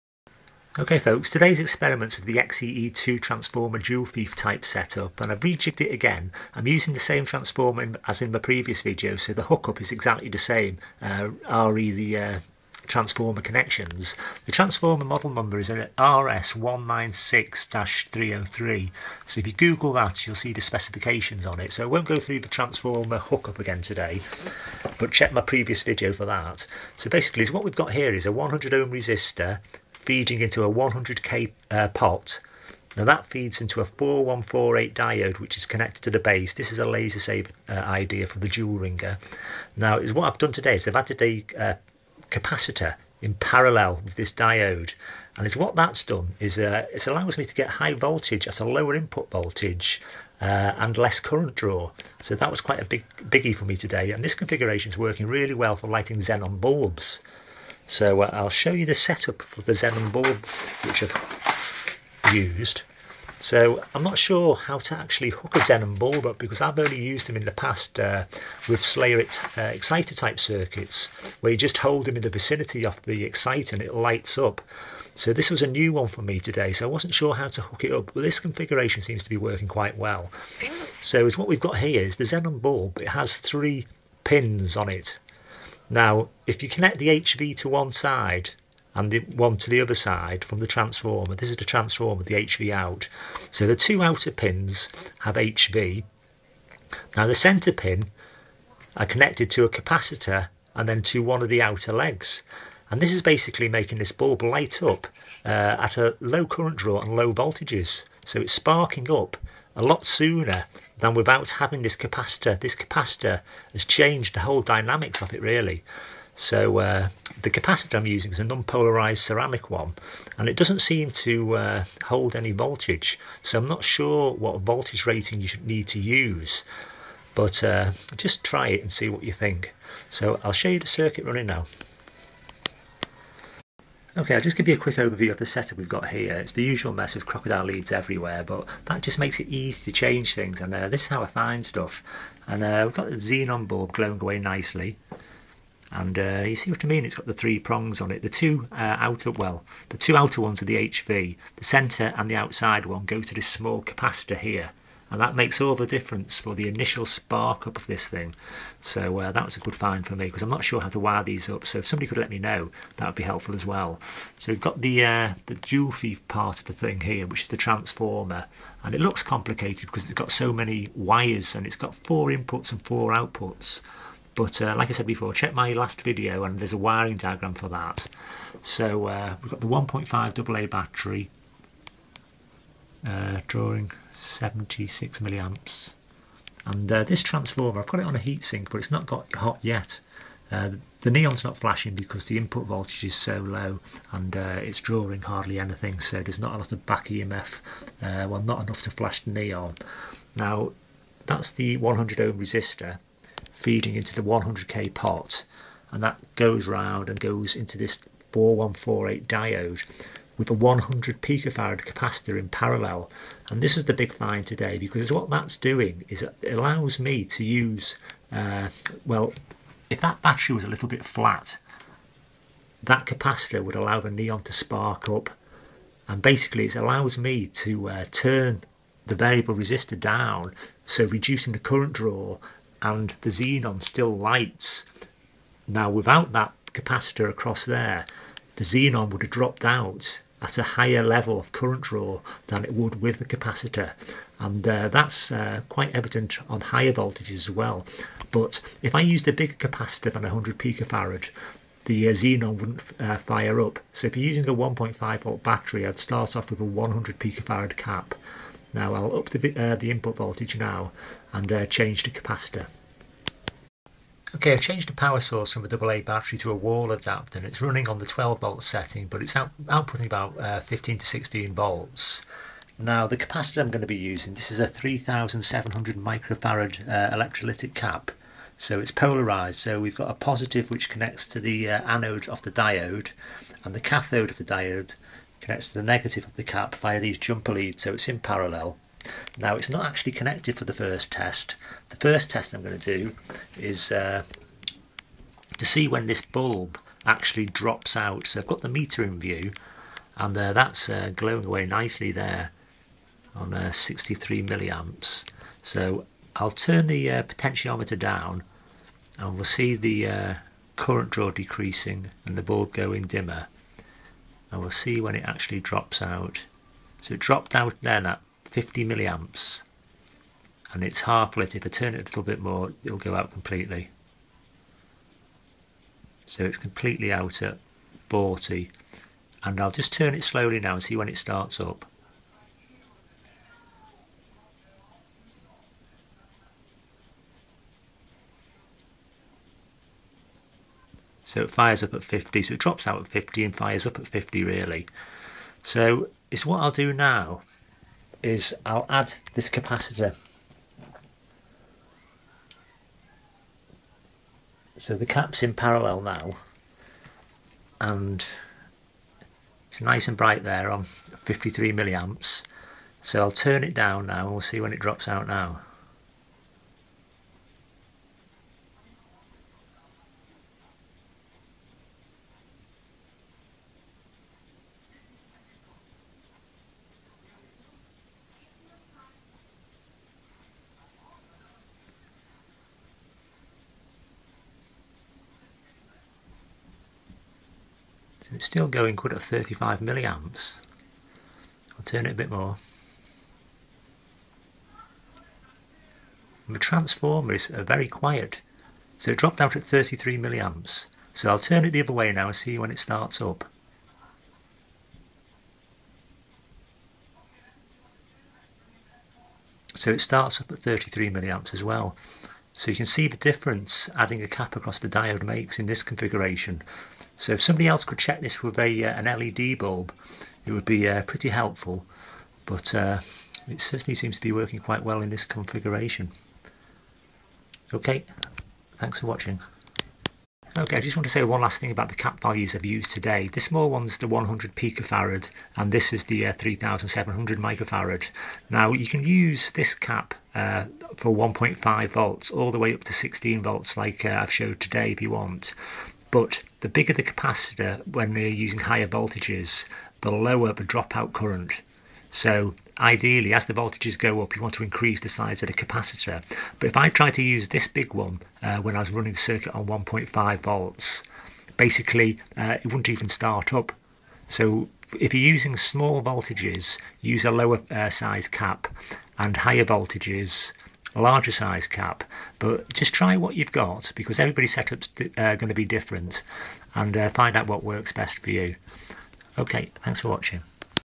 Xee2 transformer joulethief with diode/cap-xenon bulb test
This experiment is based on the Xee2 transformer joulethief.
In this test I added the Lasersaber base diode and I put a capasitor in parallel
which allowed the xenon bulb to light at a lower current draw and prevents dropout..
I used various capasitors ranging from 100pf to 3300uf.The small 100pf cap worked best for low voltage ie 1.5v and above 1.5v you can use larger caps.